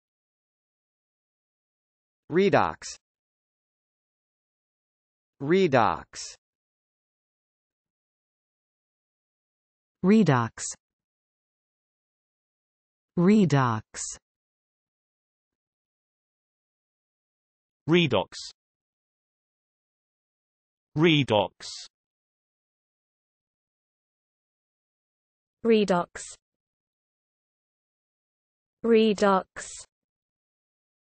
How to pronounce redox
Learn how to say Redox in English correctly with TextToSpeech.io free pronunciation tutorials. American & British English pronunciation of male & female voices provided in videos. Check them out!

    Redox definitions:
    (n): a reversible chemical reaction in which one reaction is an oxidation and the reverse is a reduction